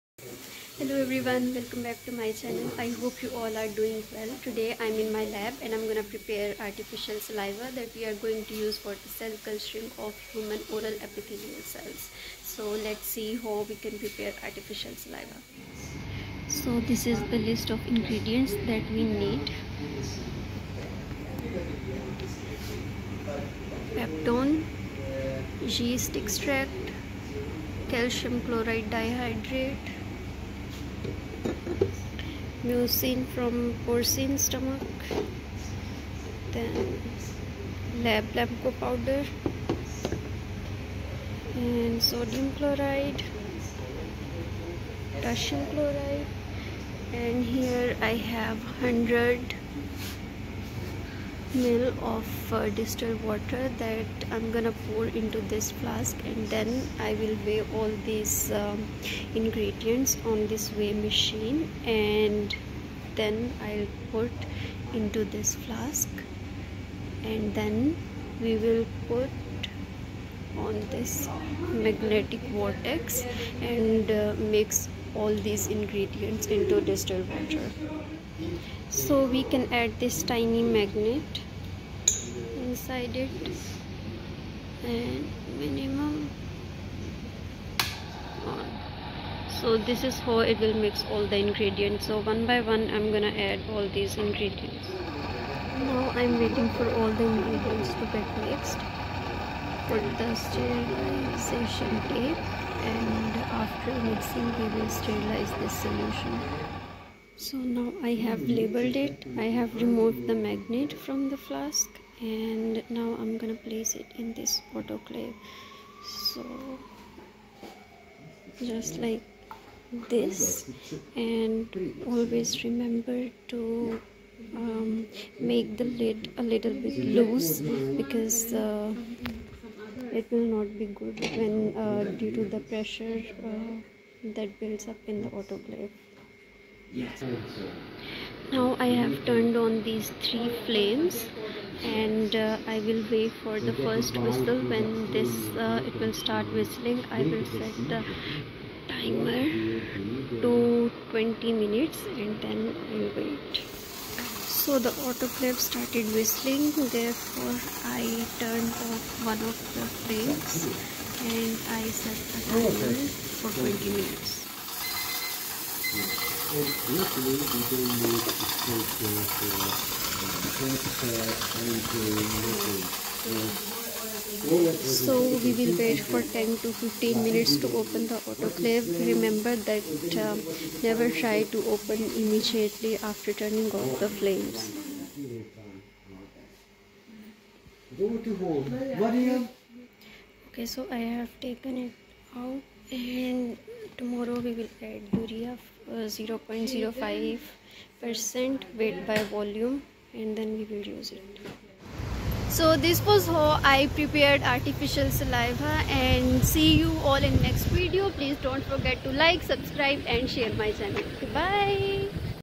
Artificial saliva preparation || Microbiology lab || Ufaq Maria
Preparation of artificial saliva for cell culturing of human oral epithelial cells.